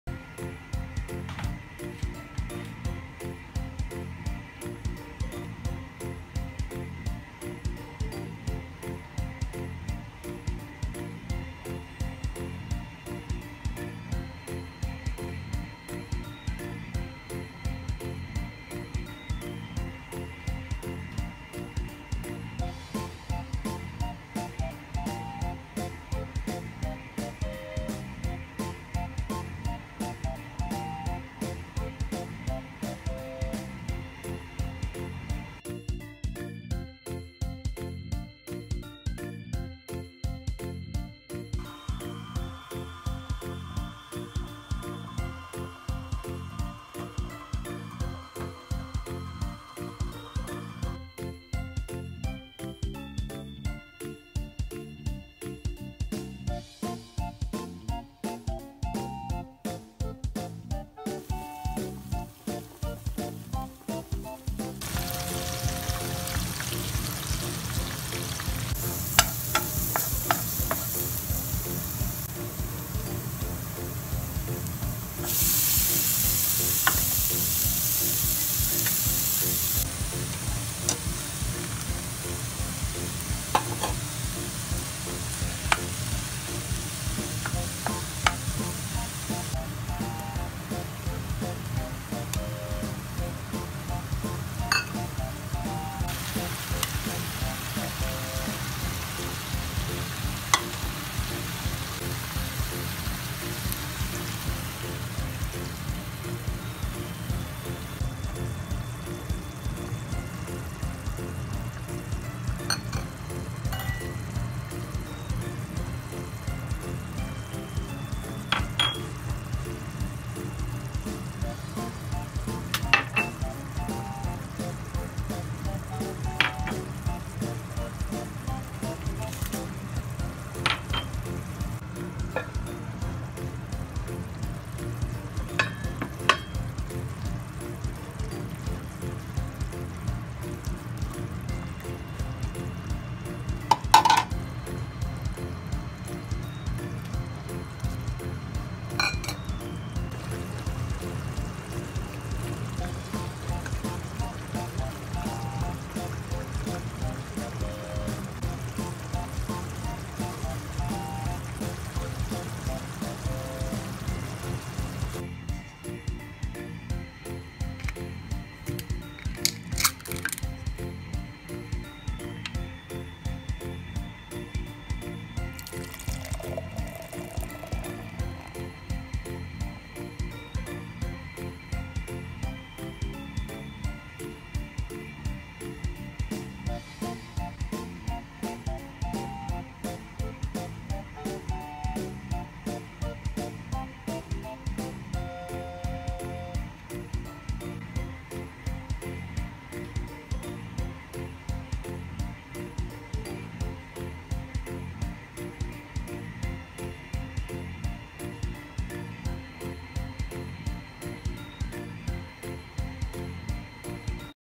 フィリピン料理🇵🇭PORK BINAGOONGAN WITH COCONUT MILK
Another delicious Filipino recipe that goes well with rice 。

Edited with Viva Video
Thank you very much for watching
Please give any comments and suggestions
Filipino dish